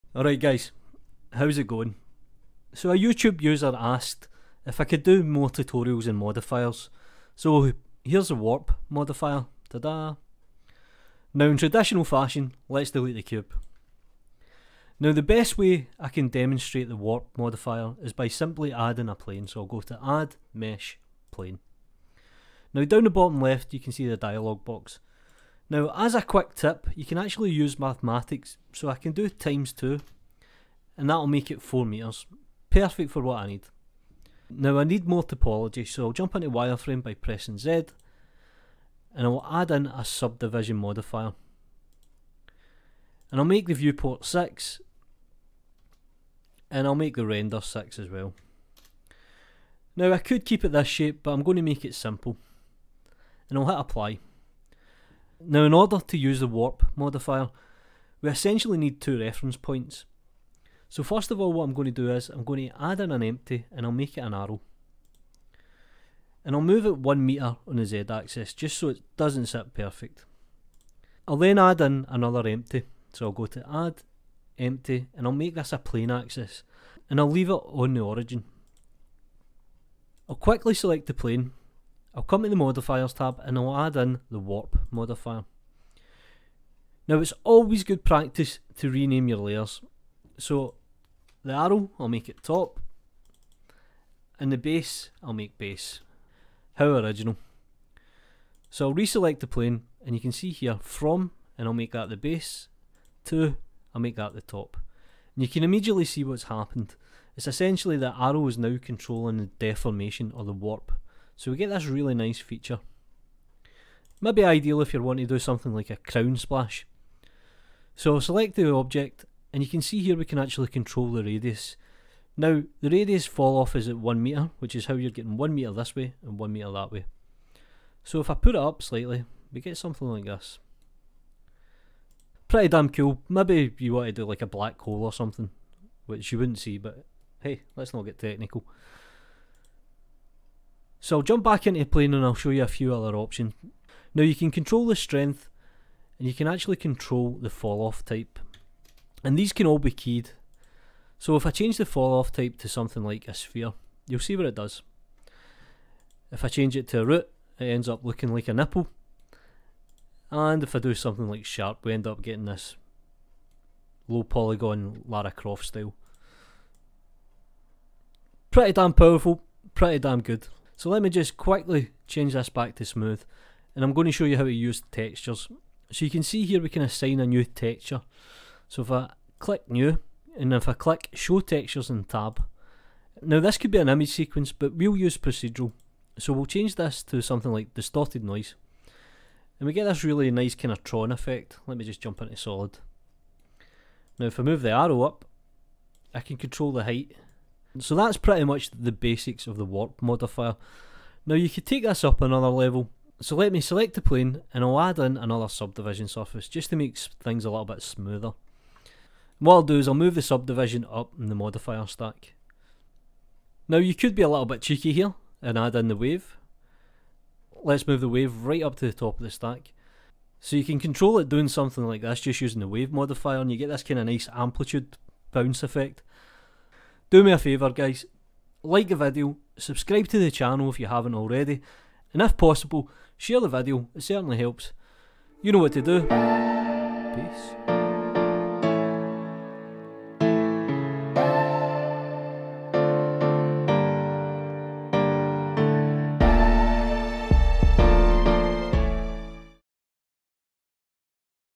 Blender 2.8 Tutorial: Warp Modifier
An introduction to Blender Warp Modifier

Warp Modifier
The Warp modifier can be used to warp parts of a mesh to a new location in a very flexible way, by using two objects to select the “from” and “to” regions, with options for using a curve falloff, texture and/or vertex group.